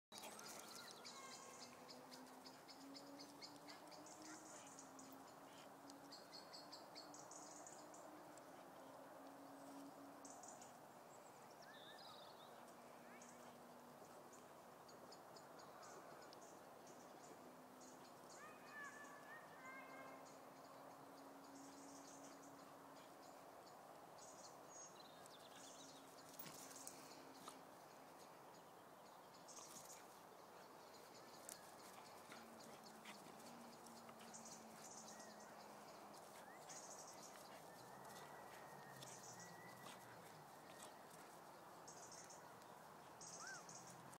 Dizzy betty
Betty getting a wee bit dizzy and confused at the park and you can see her stagger at the end too MWAAA HAAA HAAA HAAA HAAA